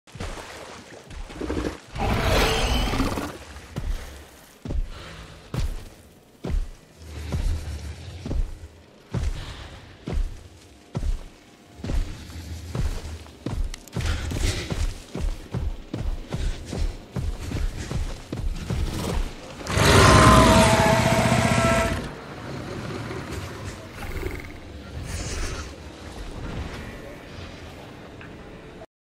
My resting spot! 😤
The new scorpios Rex is probably my favourite dinosaur in the game now.